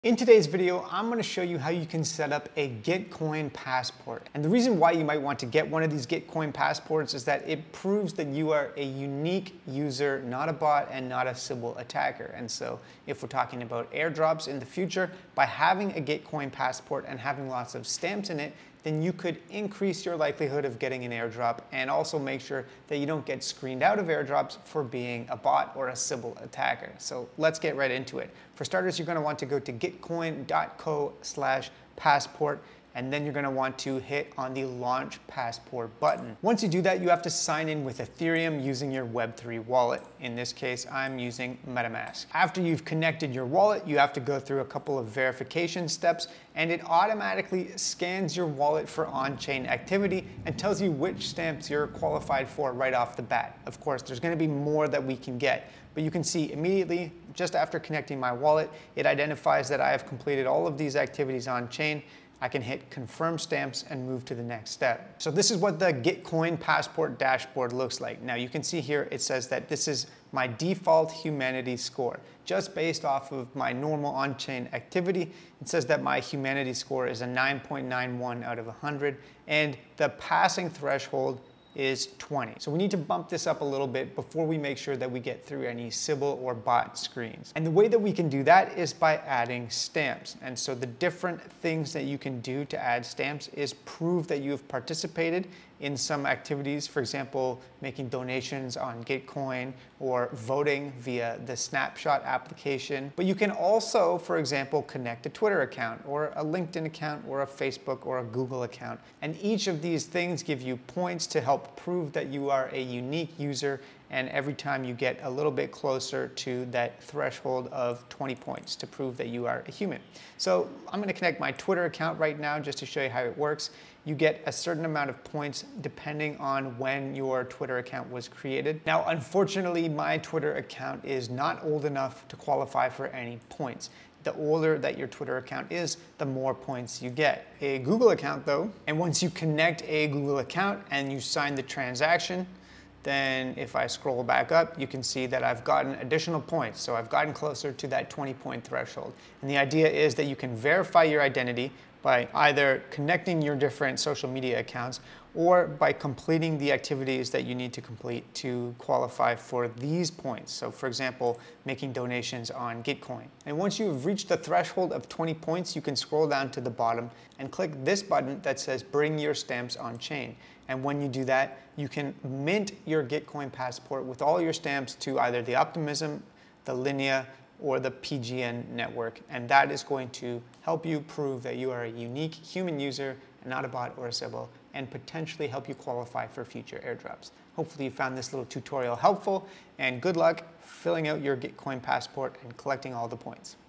Gitcoin Passport Tutorial
The Gitcoin Passport is used to prove that you are a unique, human user on the ethereum blockchain. It verifies your on-chain activity and identity and then can be used as proof that you aren't a bot or a sybil wallet.

This could be helpful for maximizing airdrops (or at least making sure that your wallets don't get screened out of airdrops).

Good luck!

My public wallets:
Ethereum: kryptocove.eth
Solana: kryptocove.sol